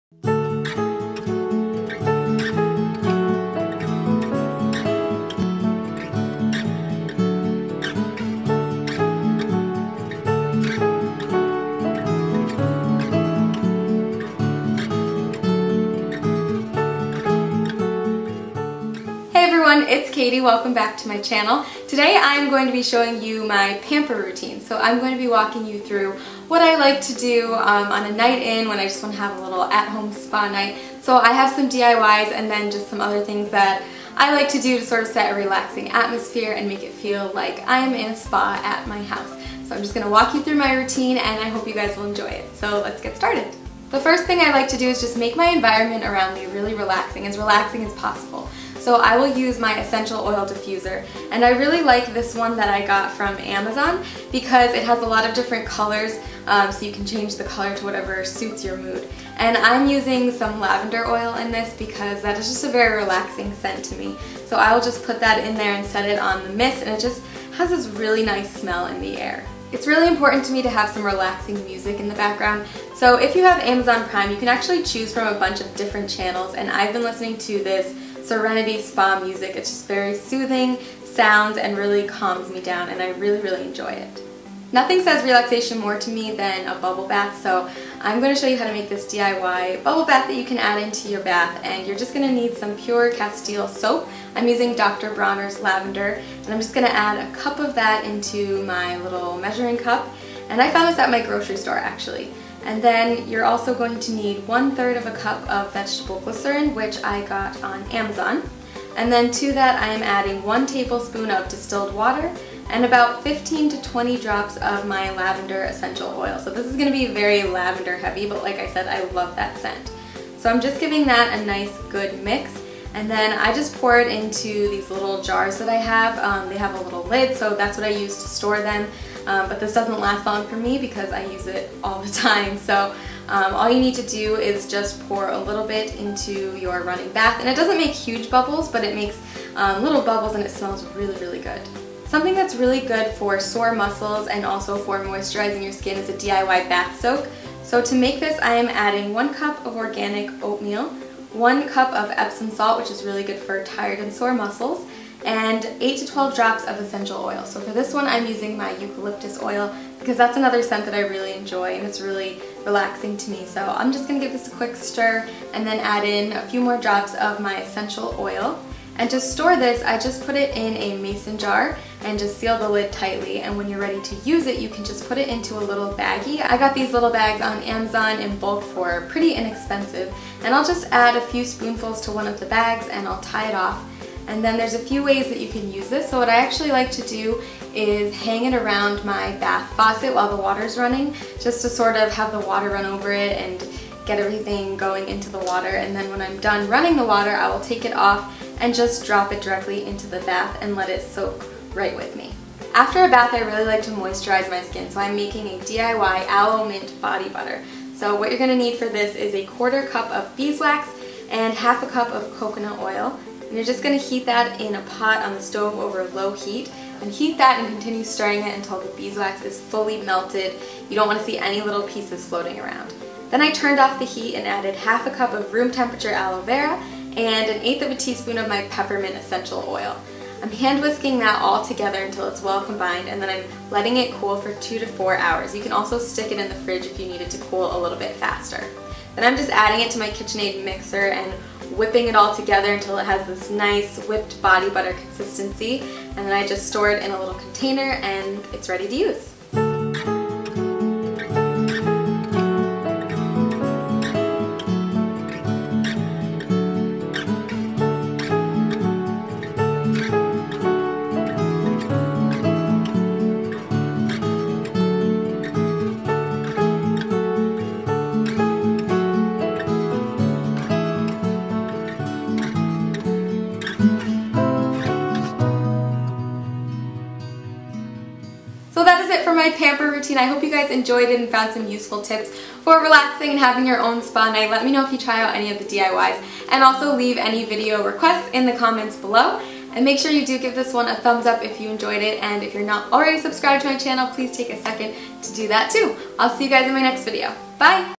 SPA NIGHT DIYS + PAMPER ROUTINE || DIY Bubble Bath, Bath Soak, Body Butter
These are some of my favorite DIYs for a relaxing at home spa night. You don't have to go to a spa to treat yourself to a relaxing pamper routine. I'm showing you 3 spa DIYs for the ultimate relaxing spa night: DIY lavender bubble bath, DIY oatmeal + epsom salt bath soak, and DIY aloe mint body butter. Plus some of my tips for creating a relaxing environment. I hope you enjoy it!